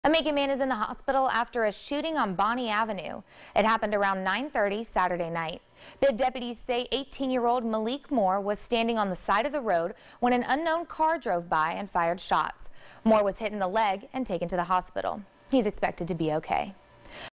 Shooting On Bonnie Avenue Leaves a Man in Hospital
A Macon man is in the hospital after a shooting on Bonnie Avenue.